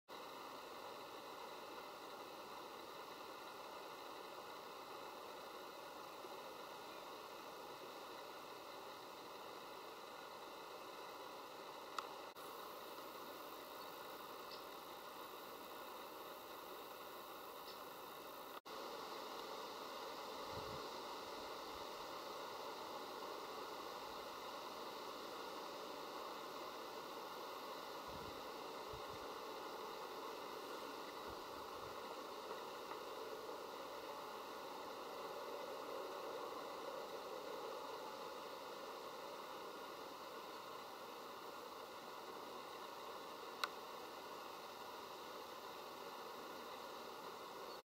Modrásek hořcový - Maculinea alcon
Modrásek hořcový - na videu uvidíte modrého samečka, hnědou samičku a samičku kladoucí vajíčko.